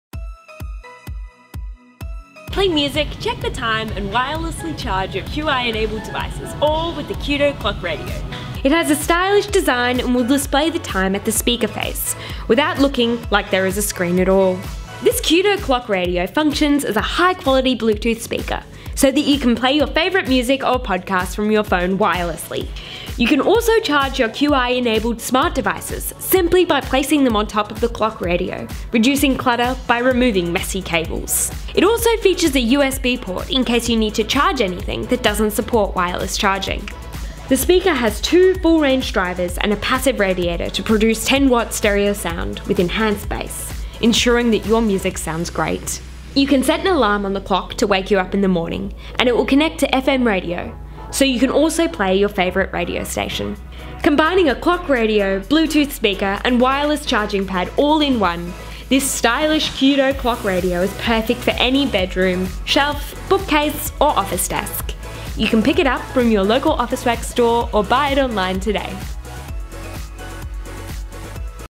Qudo Clock Radio with Wireless Charging Overview
This Qudo Bluetooth Speaker comes with a clock function so you can easily check the time and supports wireless charging of your Qi-enabled devices. It'd be perfect for keeping on a shelf in your workspace or next to your bed at home for easy access.